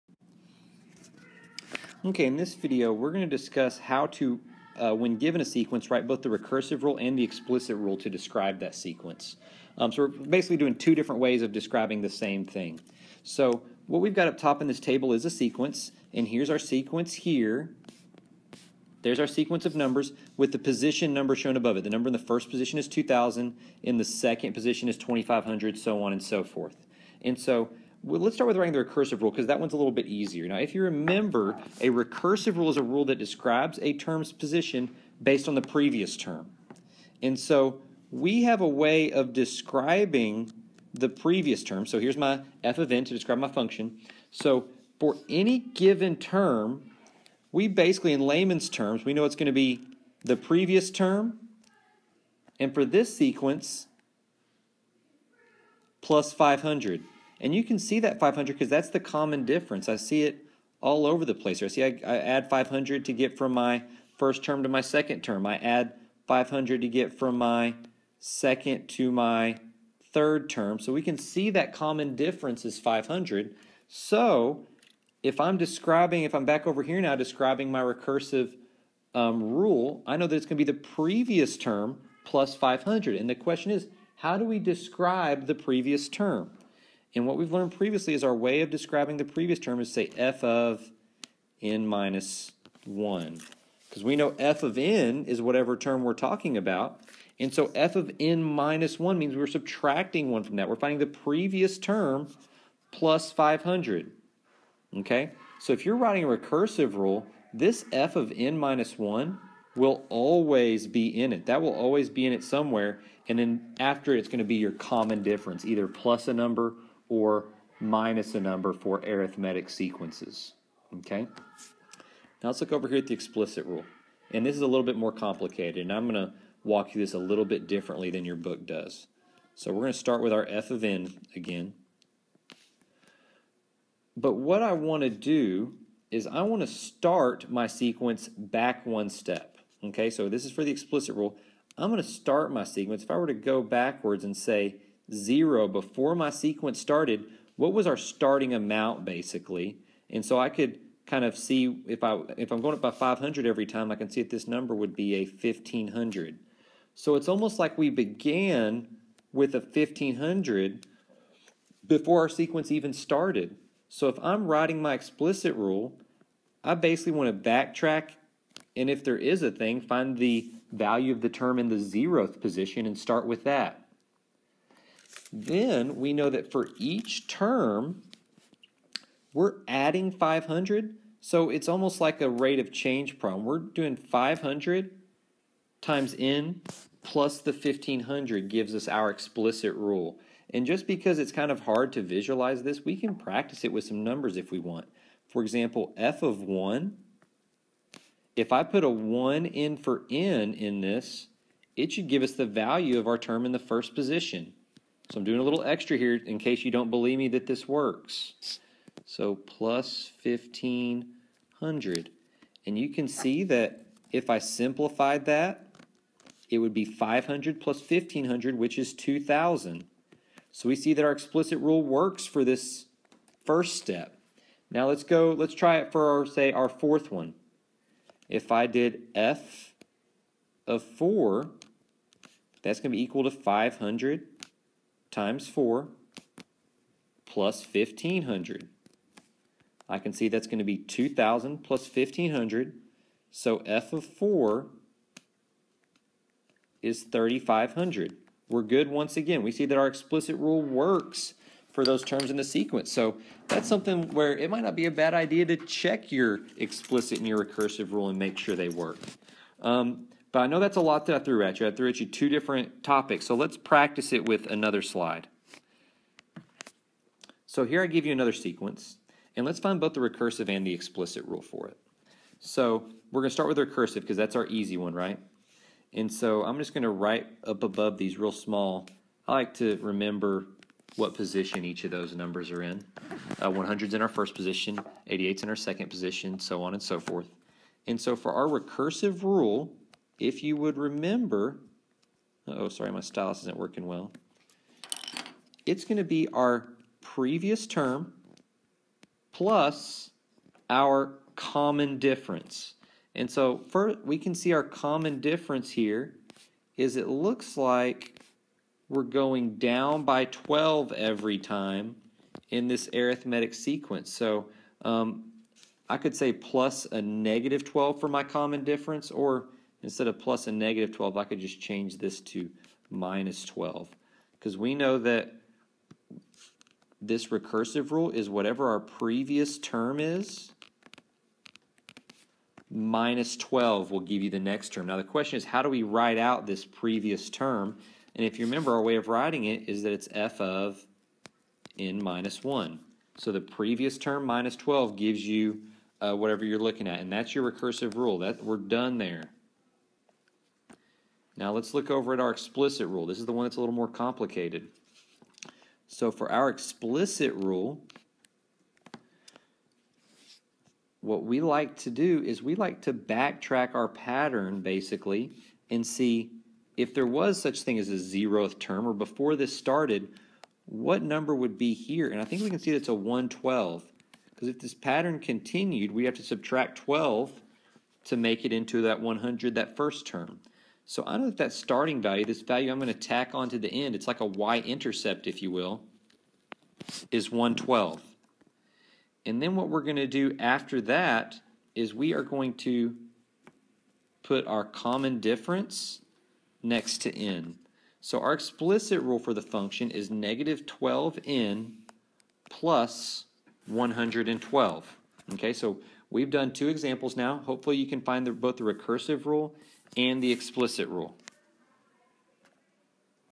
Explicit and Recursive Rules for Arithmetic Sequences
In this video, we examine how to write the explicit and recursive rules of arithmetic sequences.

*There is more than one way to write the explicit rule for an arithmetic sequence. In this video, we use a form that is representative of the slope-intercept form of a linear function.

TIMESTAMPS:
0:00 Example 1 - Recursive rule for arithmetic sequence
2:02 Example 1 - Explicit rule for arithmetic sequence
4:24 Example 2 - Recursive rule for arithmetic sequence
5:51 Example 2 - Explicit rule for arithmetic sequence